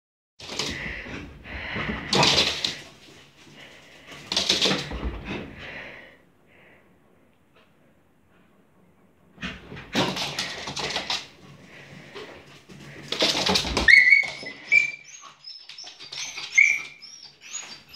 Corgi and sofa-bed
Corgi has crashed on the sofa-bed. Now he is fine, he didn't break his leg.